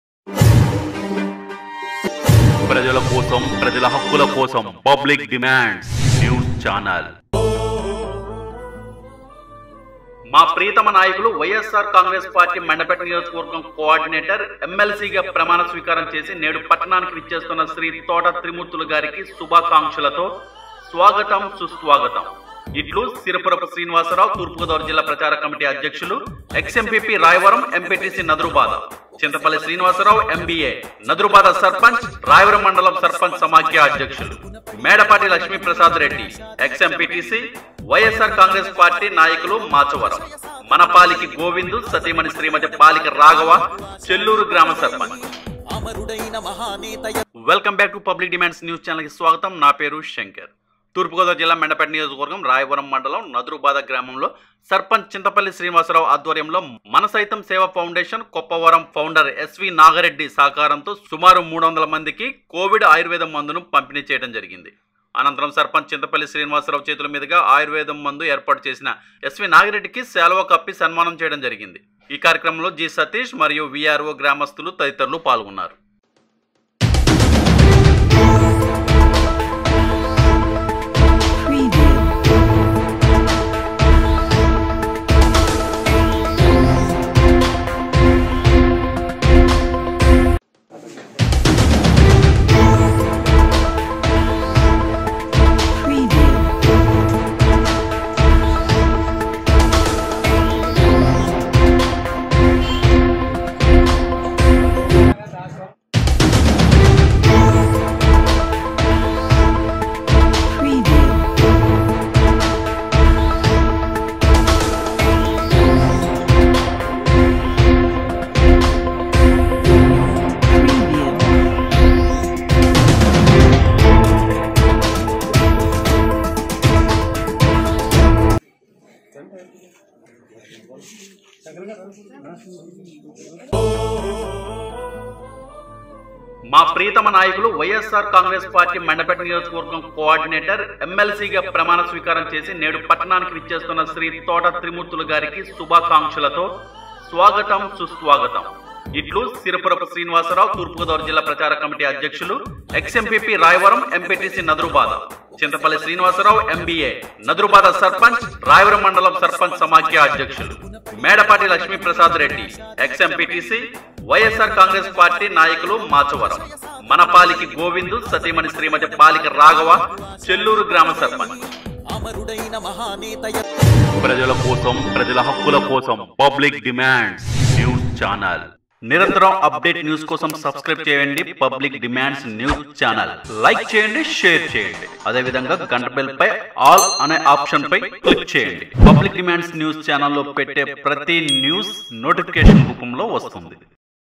మనం సైతం సేవా ఫౌండేషన్ సహకారంతో సుమారు 300 మందికి కోవిడ్ ఆయుర్వేద మందు PUBLIC DEMANDS NEWS
తూర్పు గోదావరి జిల్లా మండపేట నియోజకవర్గం రాయవరం మండలం. నదురుబాధ గ్రామంలో సర్పంచ్  చింతపల్లి శ్రీనివాసరావు  ఆధ్వర్యంలో మనం సైతం సేవా ఫౌండేషన్౼ కోప్పవరం (ఫౌండర్ S.V. నాగిరెడ్డి )  సహకారంతో సుమారు 300 మందికి కోవిడ్  ఆయుర్వేద మందు ను  పంపిణీ చేయటం జరిగింది. అనంతరం  సర్పంచ్ చింతపల్లి శ్రీనివాస రావు చేతులమీదుగా  ఆయుర్వేదం మందు ఏర్పాటుచేసిన  S.V. నాగిరెడ్డి కి శాలువా కప్పి సన్మానం చేయడం జరిగింది కార్యక్రమంలో  G. సతీష్ , మరియు  VRO   గ్రామస్తులు పాల్గొన్నారు.

     ꧂Thankyou ꧂